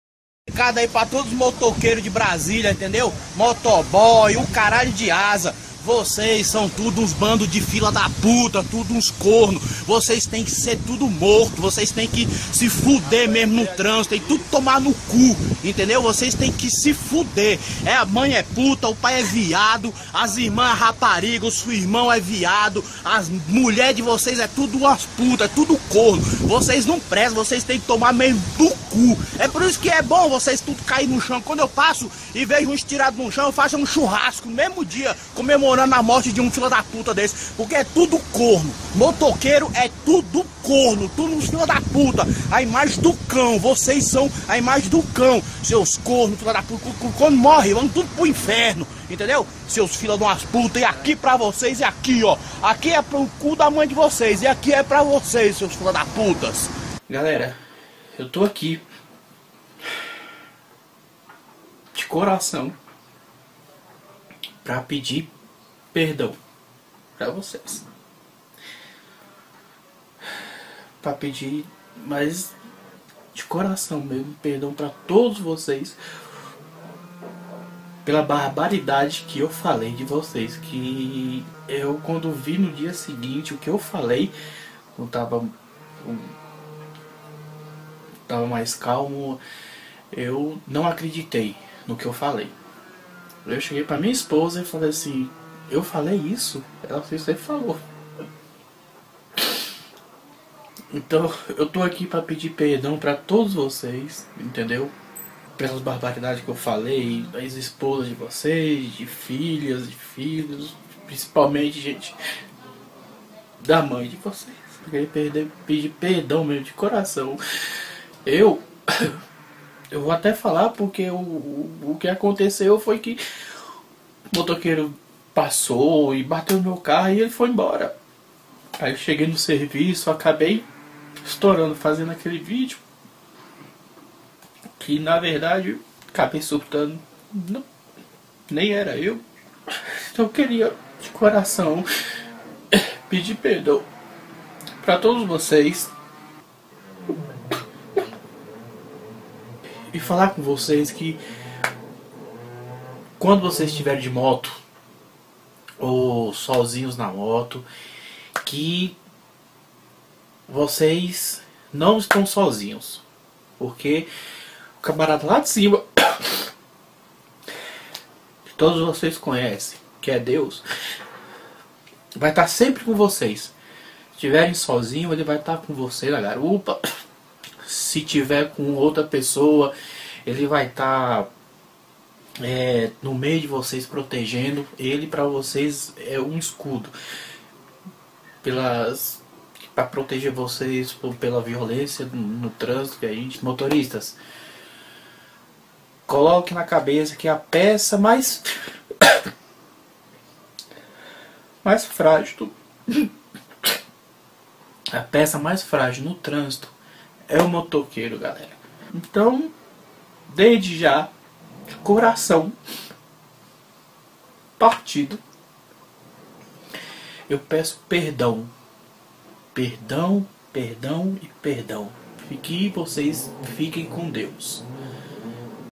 Revoltado com os motoqueiros de brasília se arrepende :)
esse ai nervosinho em bicho;
se arrependeu.